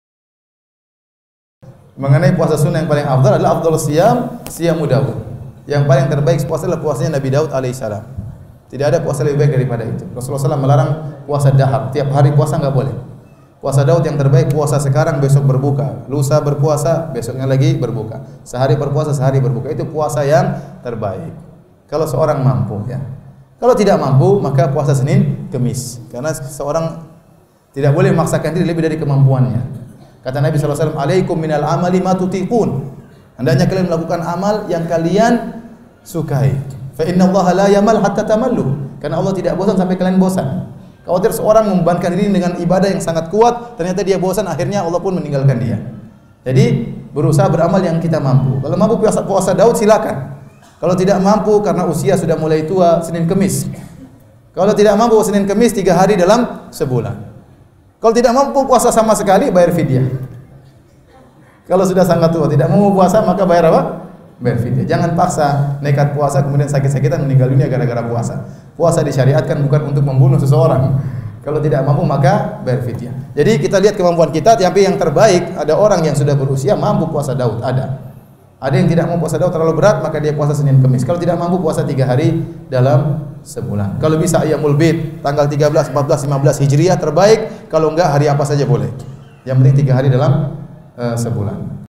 Tanya Jawab : Puasa Sunah Yang Paling Afdhol - Ustadz DR. Firanda Andirja, MA
UFA Official YouTube Channel